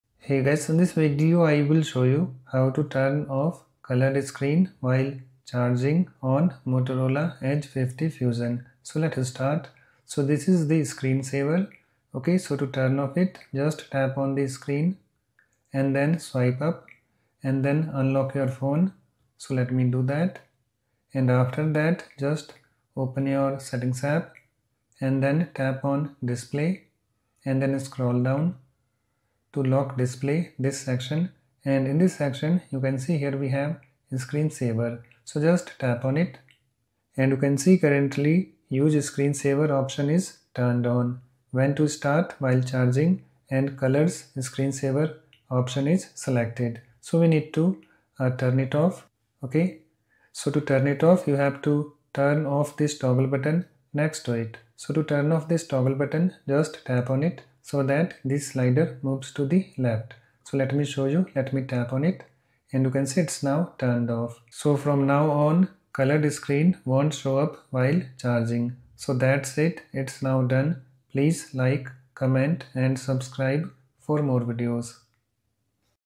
How to Turn Off Colored Screen While Charging on Motorola Edge 50 Fusion?
Hi guys, In this Video, I will show you "How to Turn Off Colored Screen While Charging on Motorola Edge 50 Fusion."

How to Enable Developer Options on Motorola Edge 50 Fusion?